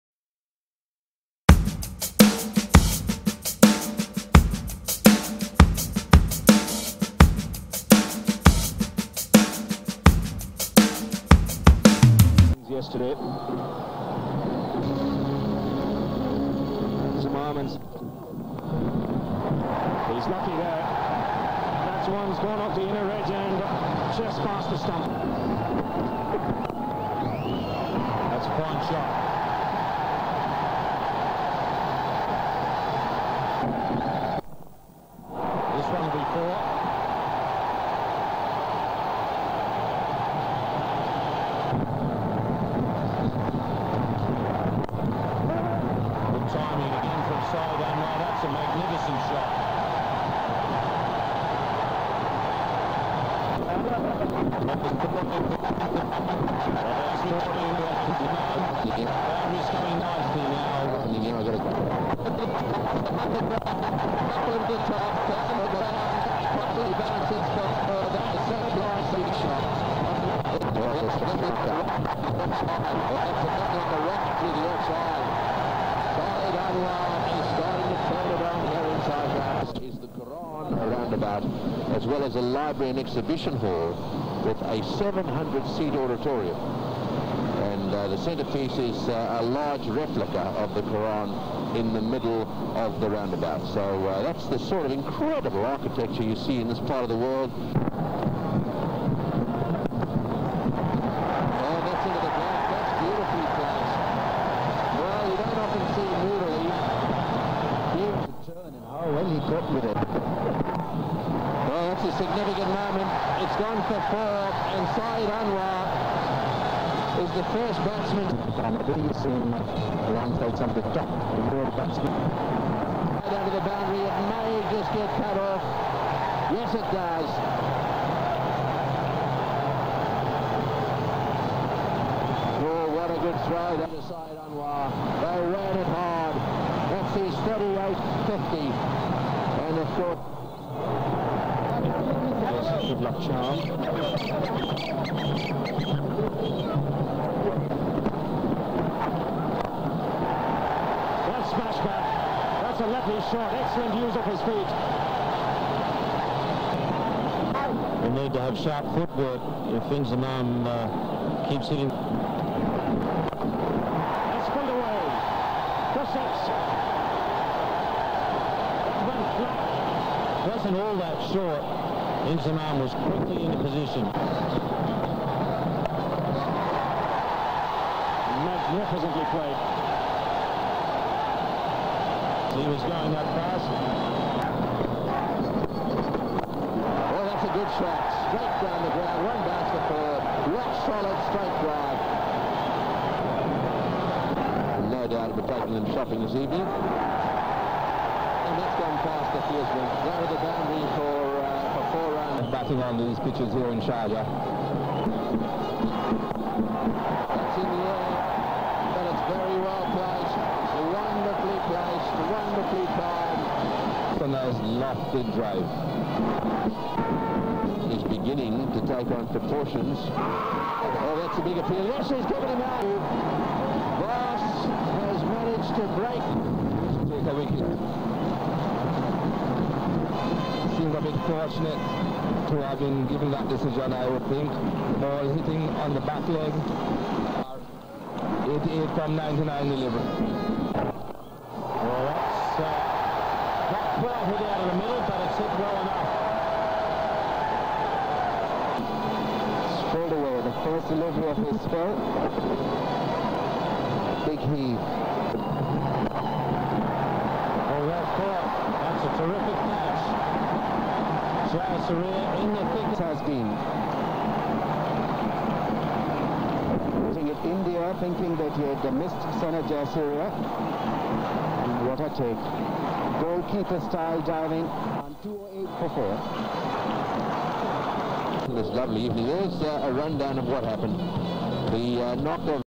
Saeed Anwar 88 | Inzamam-ul- Haq 87 | Both Reach 2K Runs on this Sharjah Ground | Ary Gold Cup 2001
Saeed Anwar & Inzamam-ul-Haq Pakistan Best batsmen's in 90s & 2000 era completed their 2000 runs in Sharjah ground ONTHISDAY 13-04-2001 in the same Game vs Srilanka. Anwar 1st to reach Scored (88) & @Inzamam08 (87) also 178 stand for 3rd wicket. Pak won that game by 28 runs.